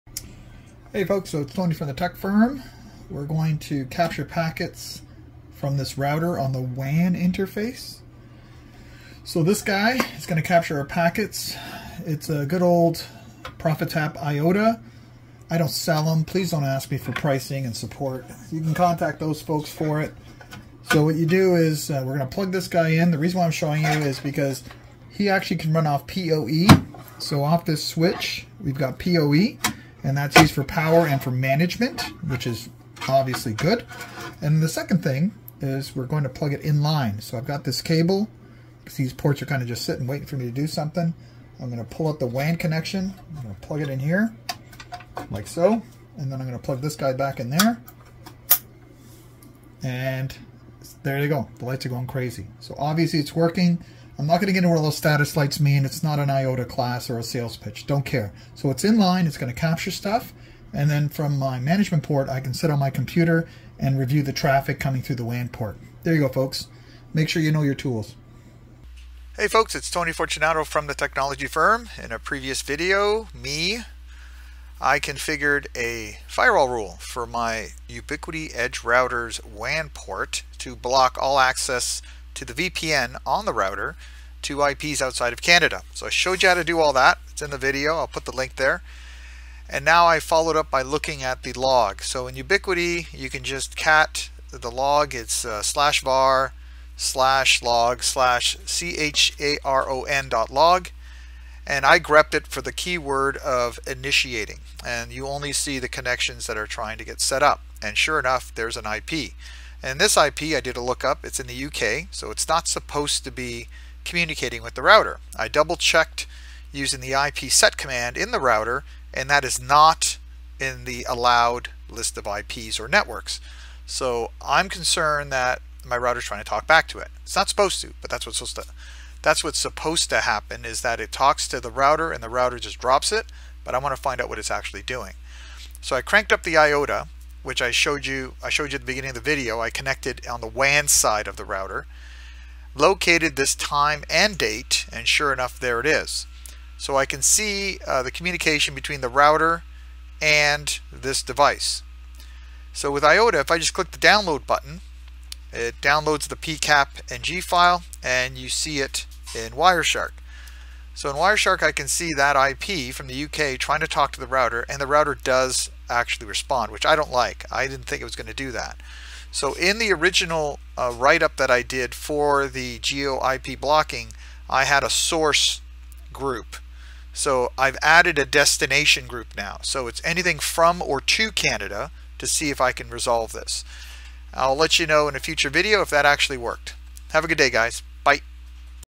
Ubiquiti Geo Ip Blocking Follow up
Verifying changes to your firewall after making modifications is crucial for maintaining the security and functionality of your network. Firewalls serve as the first line of defense against unauthorized access, so it's essential to ensure that any rules or configurations you've implemented are actively working as intended. Simply assuming that changes have taken effect without confirmation can leave your system vulnerable to threats or cause disruptions in legitimate traffic.